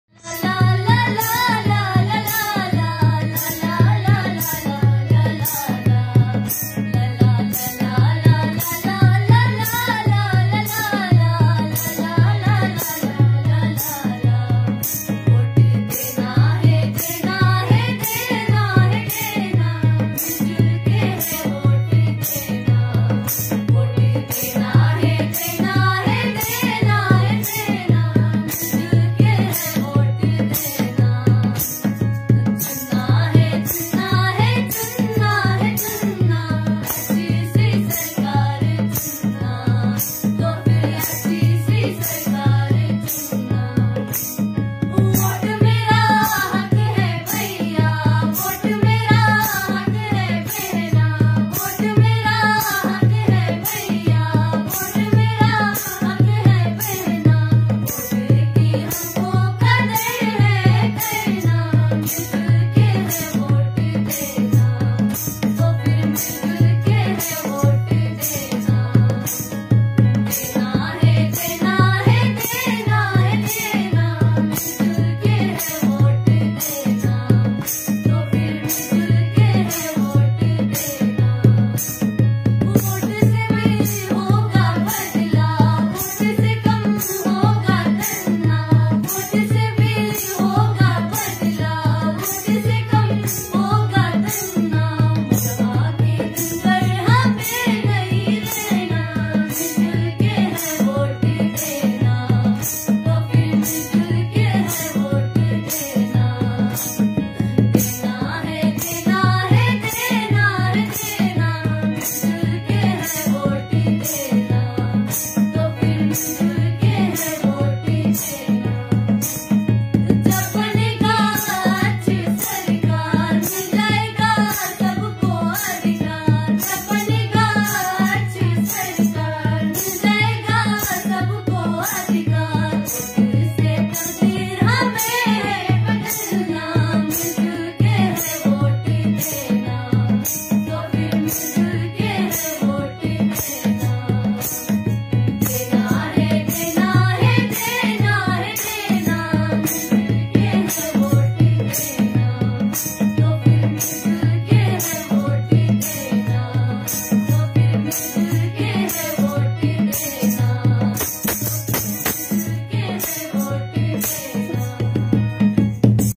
GOVT High secondary school Bhimbet Drass /voters day/ beautiful song in ladakhi language
voters day